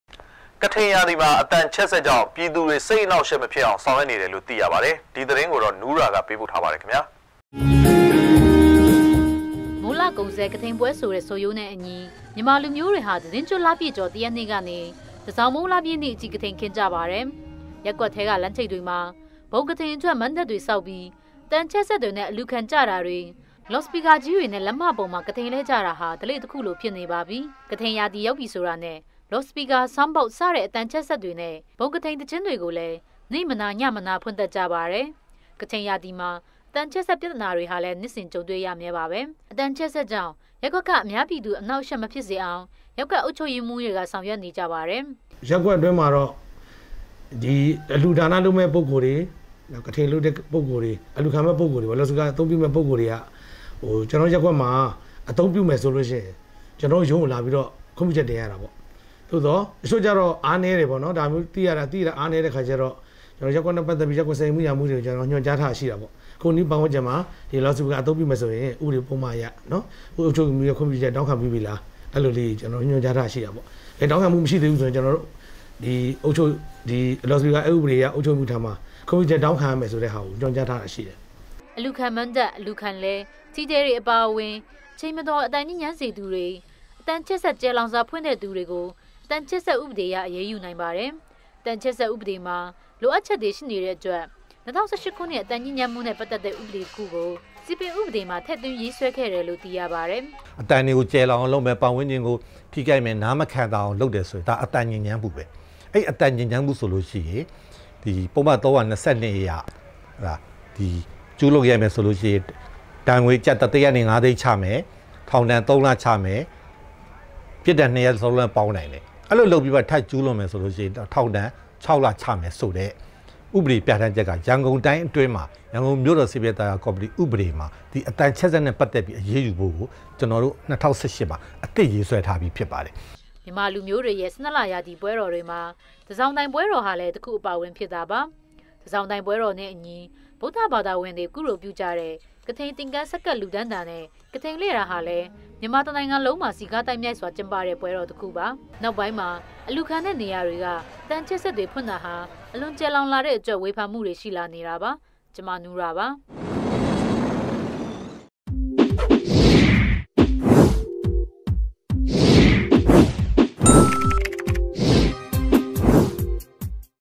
DVB - ကထိန္ရာသီမွာ အသံခ်ဲ႕စက္ေၾကာင့္ စိတ္အေႏွာင့္အယွက္ မျဖစ္ေအာင္ ေဆာင္႐ြက္ေနတယ္
ကထိန်ရာသီမှာ အသံချဲ့စက်ကြောင့် ပြည်သူတွေ စိတ်အနှောင့်အယှက် မဖြစ်အောင် ဆောင်ရွက်နေတယ်လို့ သိရပါတယ်။ ဒီသတင်းကိုတော့ နူးရာက ပေးပို့ထားပါတယ်။
DVB TV -